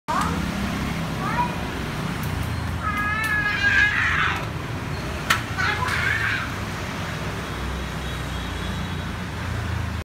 Két Truly HQ model TL50C,Giá 4,5tr
Cao 5 tấc,ngang 4,2 tấc,sâu 4,7 tấc.
Nặng 85ky.